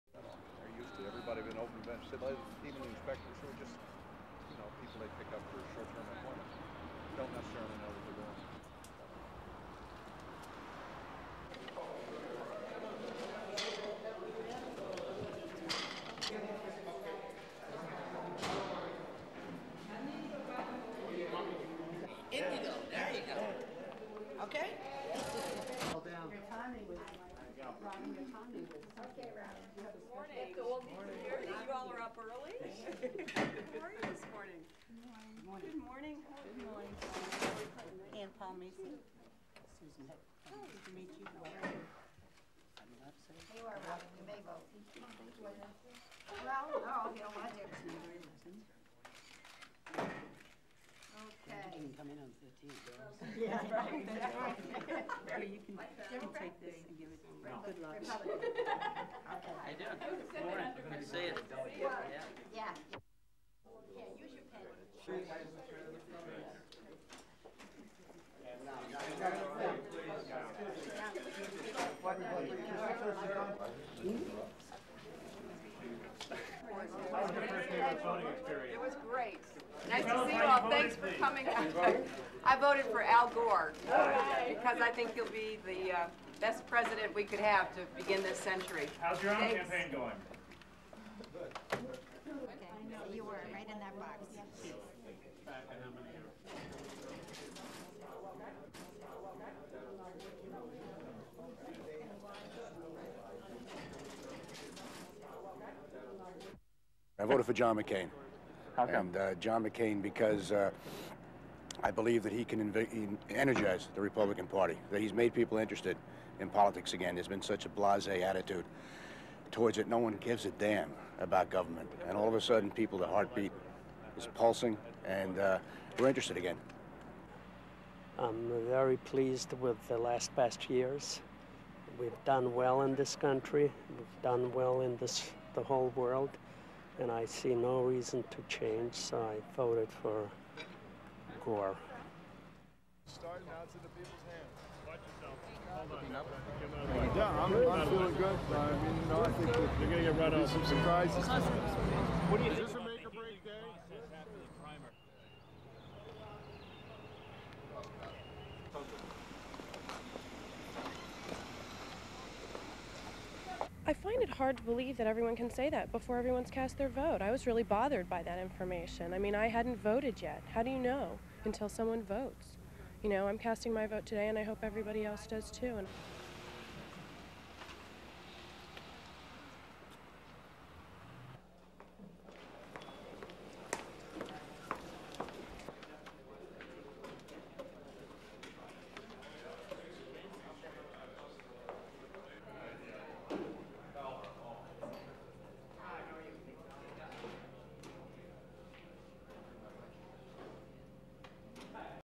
USA: PRESIDENTIAL ELECTION PRIMARIES: SUPER TUESDAY
What's known in the U-S as Super Tuesday is a defining day in the race for the White House.

For both Democrats and Republicans Tuesday is the busiest day yet of the primary voting season.

Voting takes place in almost every region of the nation with Republicans voting in 13 states, and Democrats voting in 15.

For candidates like John McCain and Bill Bradley, this Super Tuesday could make or break their campaigns.

America's First Lady currently involved in her own campaign within New York demonstrated her support towards presidential candidate Al Gore.

Polls opened in New York at six in the morning, and even at that early hour, voters were flocking to cast their ballots.

Aside from the big election day itself, Super Tuesday is perhaps the most important day in this year's election cycle.

With virtually every region of the nation at play, both parties will award more than half the delegates necessary to secure a nomination.

That means if all goes well for the front-runners today, the presidential election may be down to a two-man race.

Polls show Vice President Al Gore with a broad lead over former Senator Bill Bradley in each of the 15 states with democratic primaries.

The Republican race between McCain and Bush appears tighter, though surveys still show Bush with an advantage in some large and important states.

New York and California are the big prizes this Tuesday.

McCain and Bush spent their final moments campaigning for California's 162 delegates, while Bradley and Gore did the same in New York.

Vice President Gore got an early morning vote from one of New York's newest residents...the First Lady, herself, Hillary Rodham Clinton.

Mrs. Clinton moved to New York earlier this year to run for the Senate in November.

She took just 30 seconds to make her selection, and proudly told the press surrounding her, she voted for Gore.

SOUNDBITE:
"I voted for Al Gore. Cause I think he'll be the best president we could have to begin this century."
SUPER CAPTION: First Lady Hillary Rodham Clinton

On New York Upper East Side, Clinton's opponent for Senate, Mayor Rudolph Giuliani, cast his ballot for Bush.

But pollsters still say the Republican race is too close to call, and voters like this one hope the underdog prevails...

SOUNDBITE:
"I voted for John McCain...John McCain because he energized the Republican party, he's made people interested in politics again. There's been such a blase attitude towards it, no one gives a damn about government, and all of the sudden, peoples' hearbeats are pulsing, and registered again."
SUPER CAPTION: Kevin Moore/NY Voter

SOUNDBITE:
"I'm very pleased with the last two years. We've done well in this country, we've done well in the whole world, and I see no reason to change, so I voted for Gore."
SUPER CAPTION: Lew Reznik/NY Voter

Bill Bradley spent the morning shaking hands in New York...

Though he's not favored to win anywhere, he said he's not giving up hope.

SOUNDBITE:
"I'm feeling good, I think there will be some suprises today."
SUPER CAPTION: Senator Bill Bradley/Democratic Presidential Candidate

Though polls show he trails Vice President Gore in virtually every Super Tuesday state, Bradley supporters acknowledge their candidate must get at least one win in order to stay in the race.

And while analysts say this is probably Bradely's last day in the race, others urge not to give up too soon...

SOUNDBITE:
SUPER CAPTION: Amy Thompson/Maryland Voter

And the campaign for the general election could begin as soon as Wednesday.